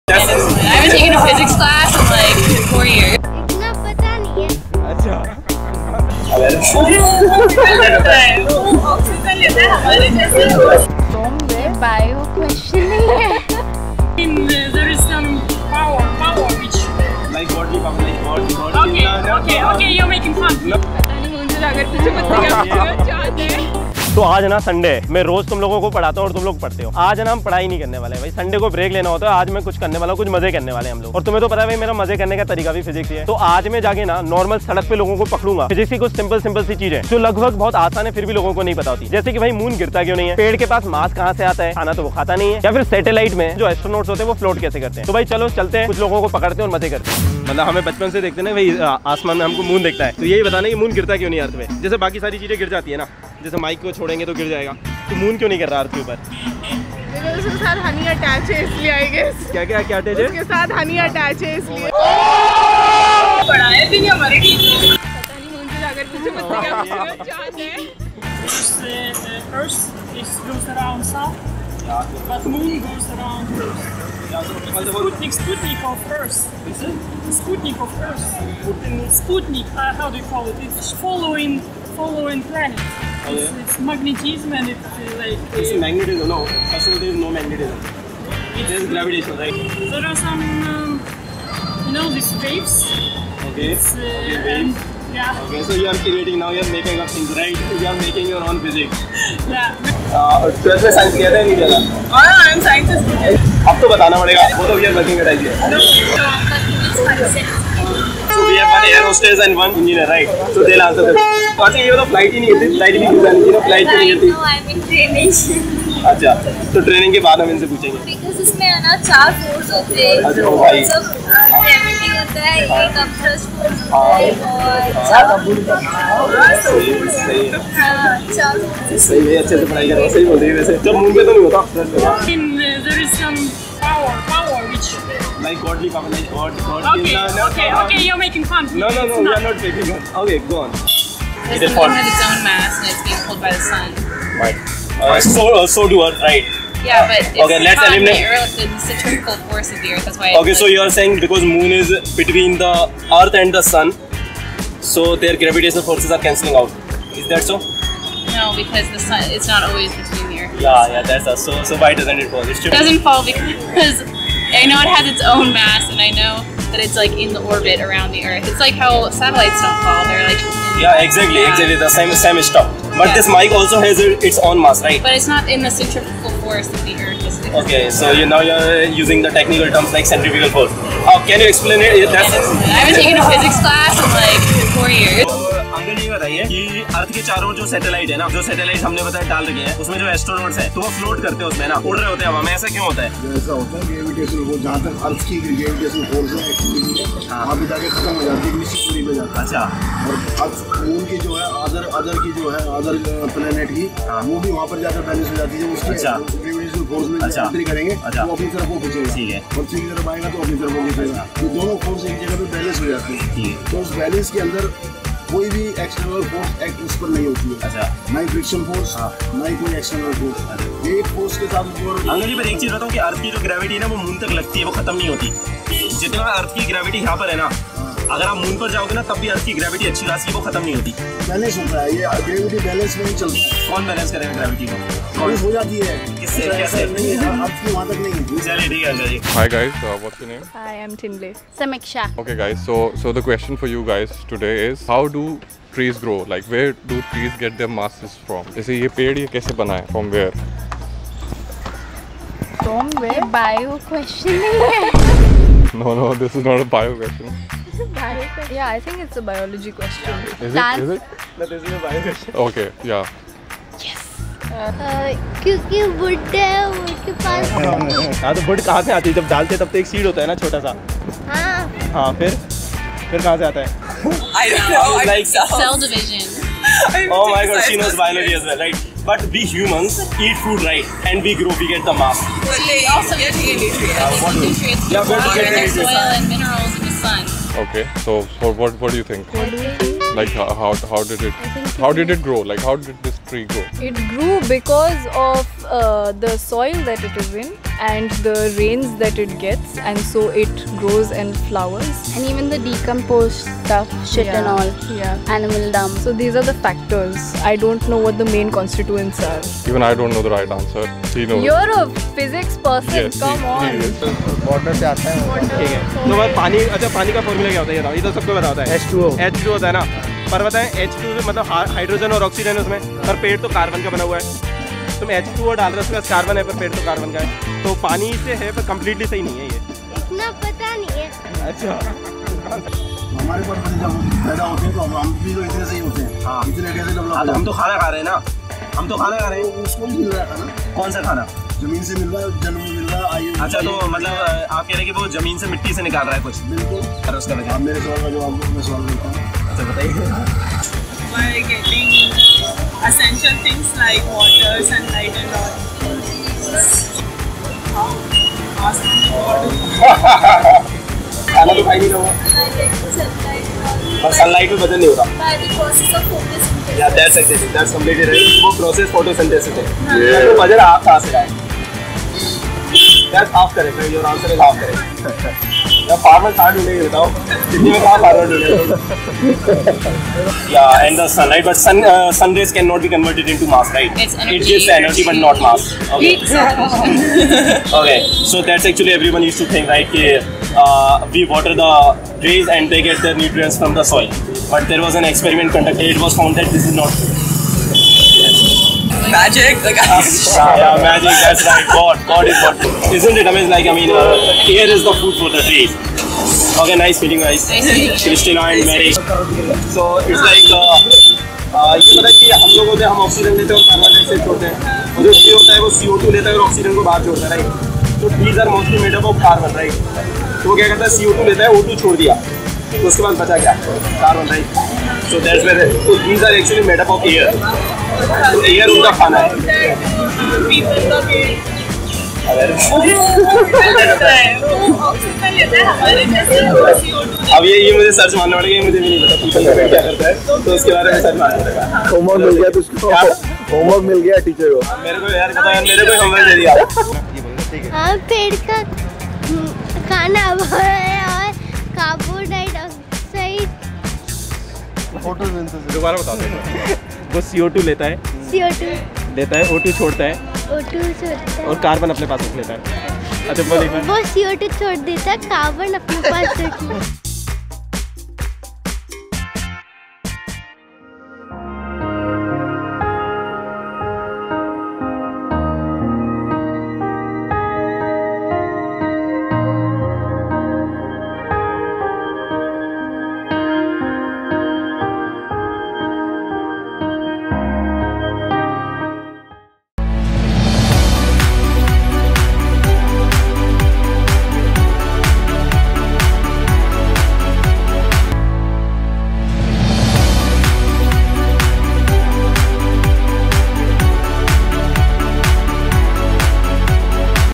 Social Experiment by a Physics FACULTY | IIT JEE / NEET | Prateek Jain Sir
An entertaining Social experiment to make people aware of few basic science phenomena like
1) Where do trees get their mass from?
2) Why moon doesn't fall?
3) How astronauts float in the satellite( ISS) even though there is sufficiently large gravity?

Soon I'll upload videos with the simple explanation of all these questions and will post their links over here.